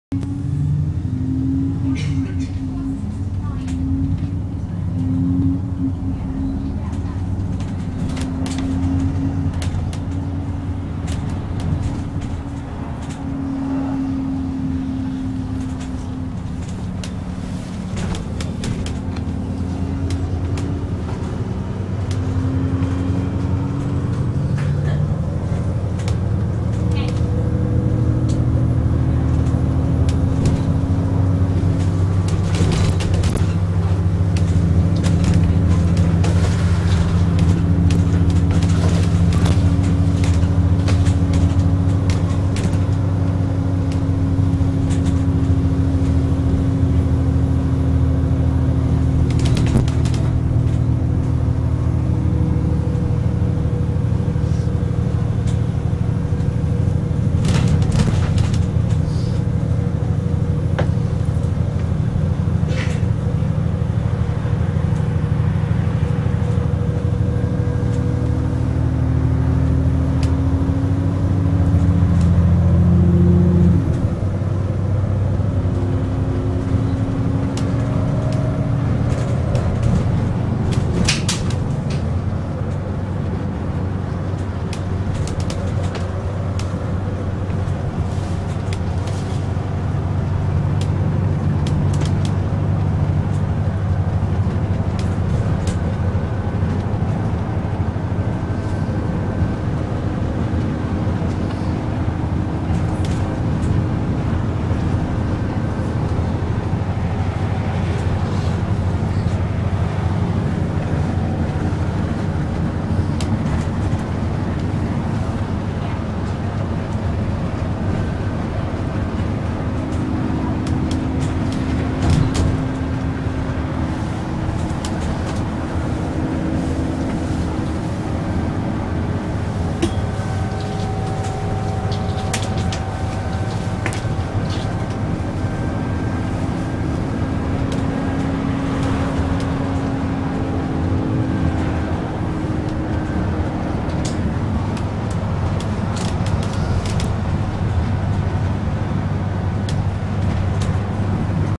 leaving cocking
went around hampshire surrey and sussex yesterday this was leaving cocking in sussex on route 60 and ikr weird name for a village lol
on
stagecoach route 60 midhurst-bognor regis